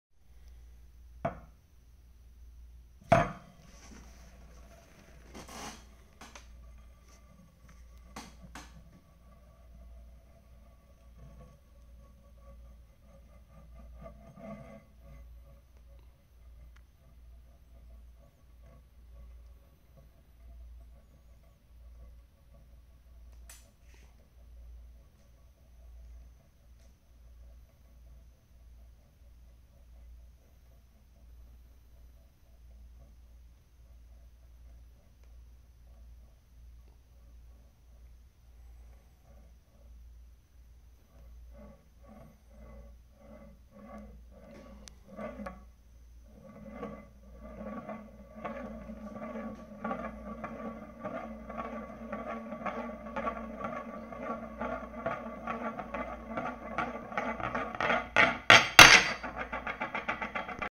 Tippe top
The tippe top I made by using a metal ring and something. The explanation for the self-inverting process is not so easy, maybe.